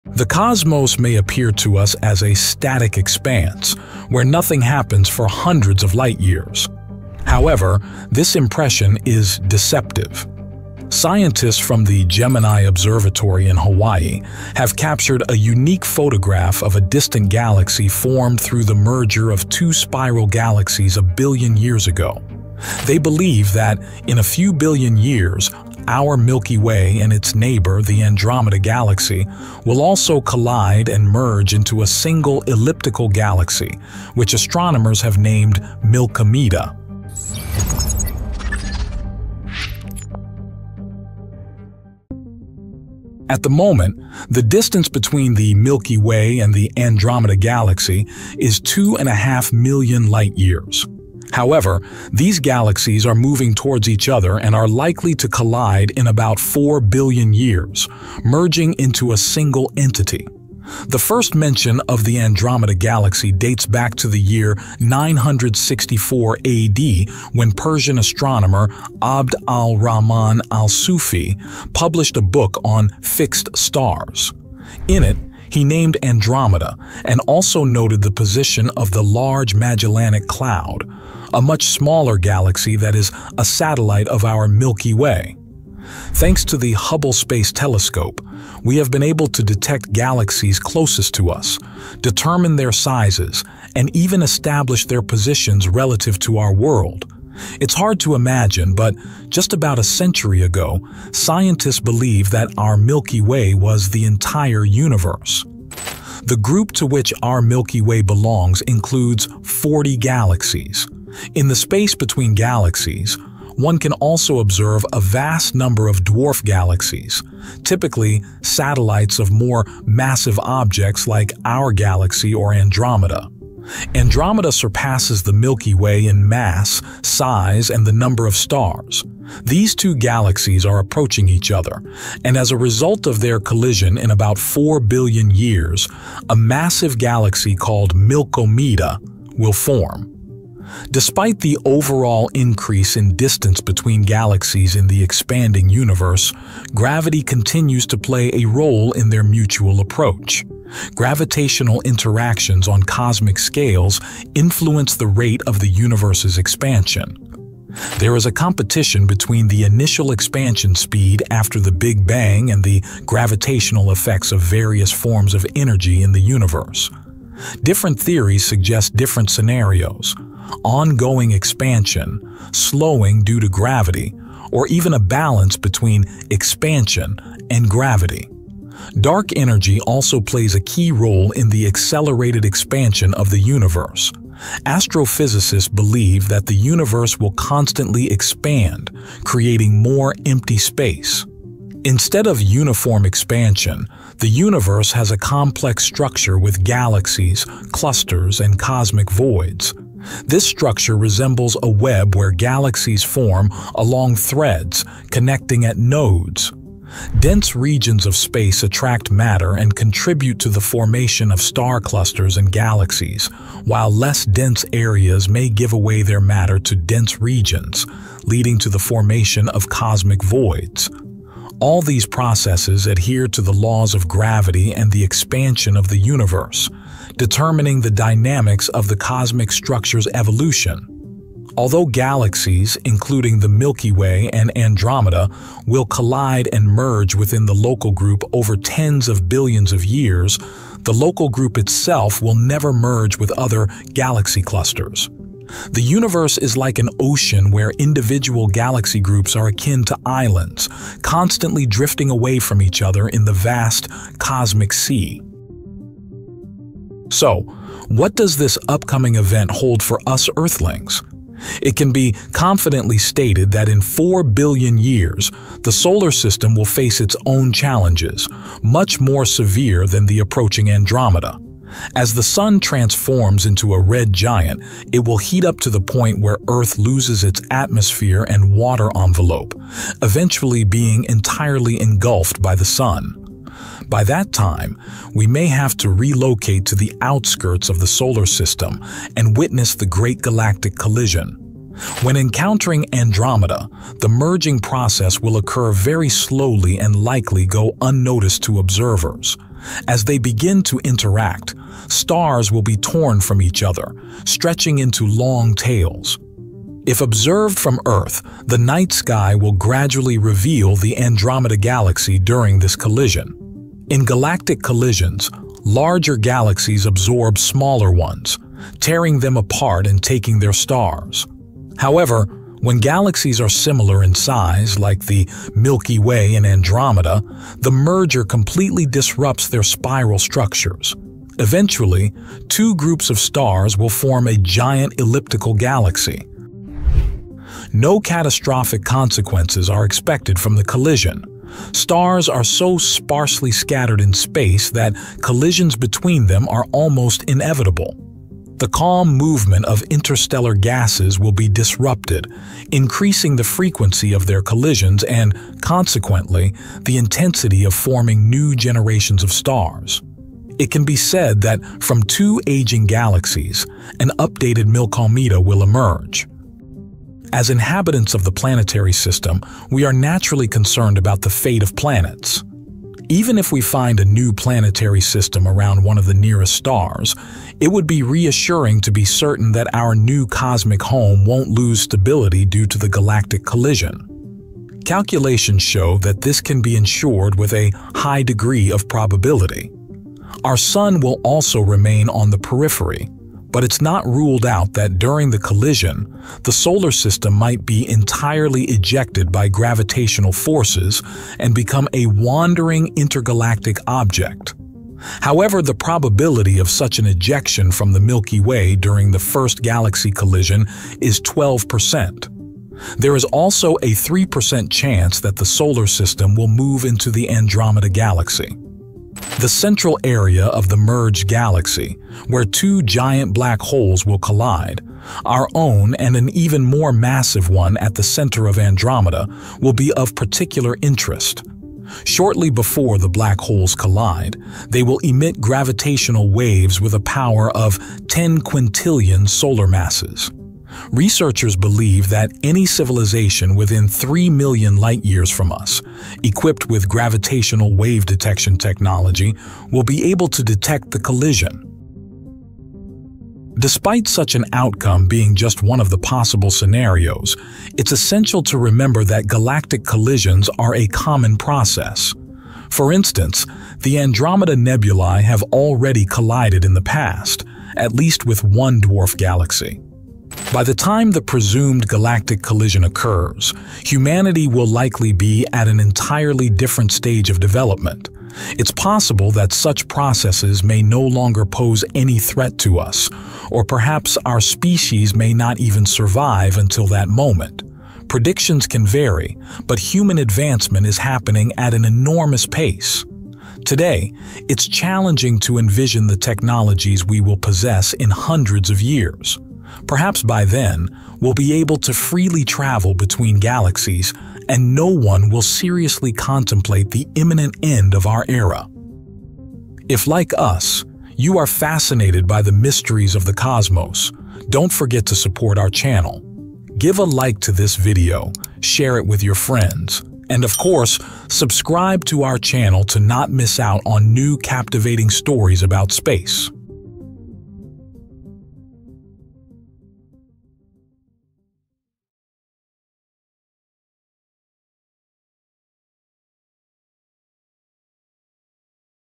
What will Happen When the Milky Way Collides with Andromeda Galaxy?
Approximately 4 billion years from now, according to scientists' predictions, a cosmic catastrophe is expected to unfold: the collision of the Milky Way and Andromeda galaxies. Following the collision of the two galaxies, researchers believe they will form a new elliptical galaxy. This merging process is estimated to last about a billion years and will be accompanied by the ejection of stars onto various orbits around a new galactic center.